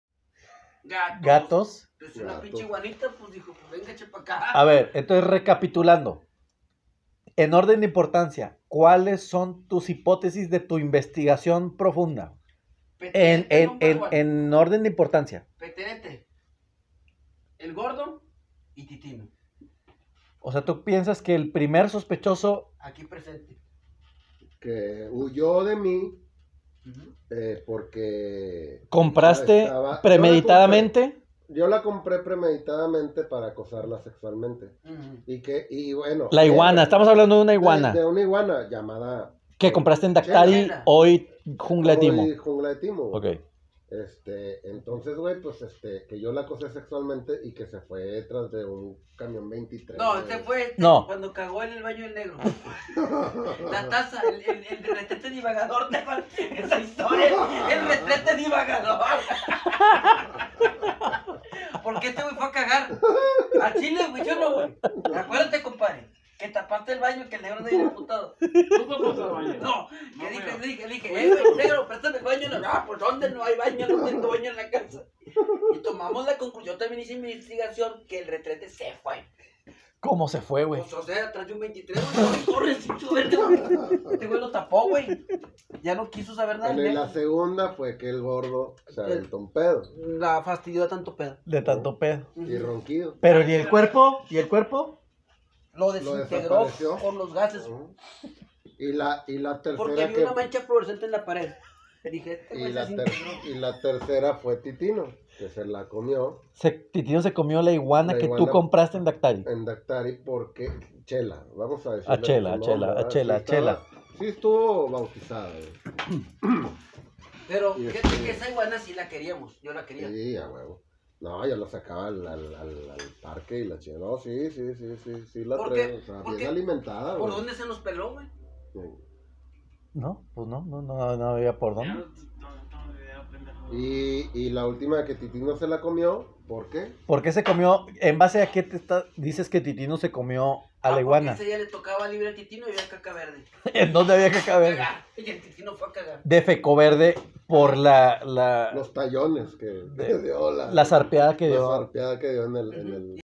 Investigación Juan Serrato
Después de varios años, la investigación realizada por Juan Serrato sale a la luz...